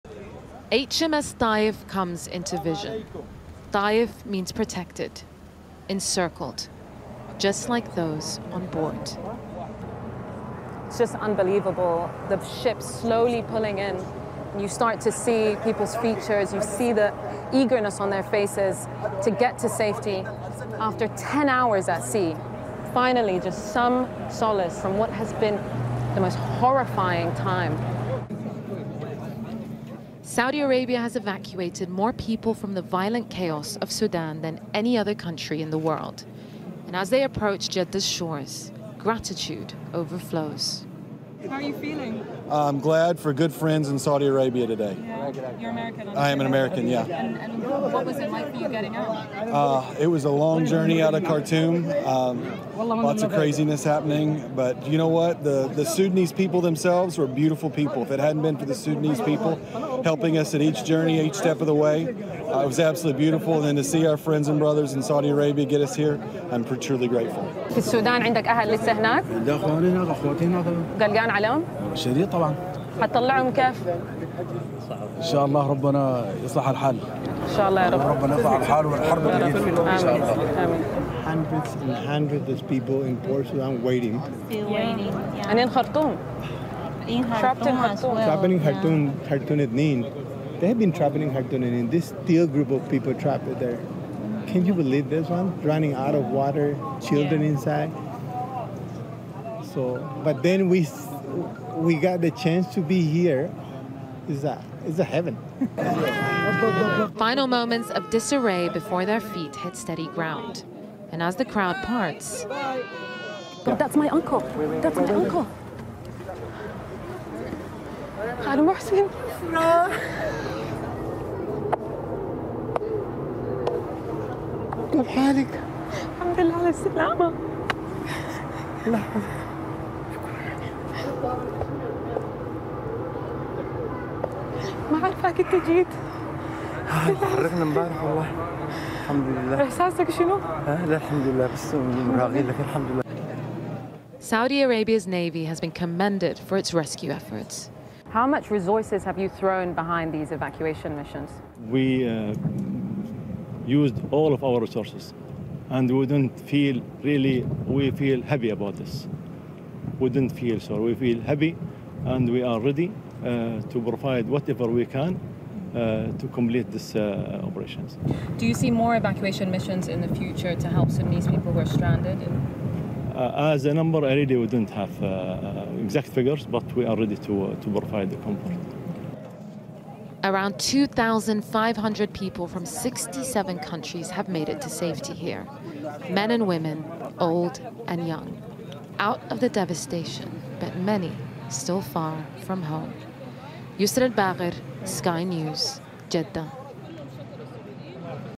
Sudan: Families embrace after finding safety in Saudi Arabia
Sky's Africa correspondent, Yousra Elbagir, watched as evacuees arrived in Saudi Arabia from Sudan on HMS Taif, a name that means protected or encircled in Arabic.

In the crowd, she spotted her uncle, Mohsin, a Sudanese-American surgeon, who boarded a ship in Port Sudan to escape the fighting in Khartoum.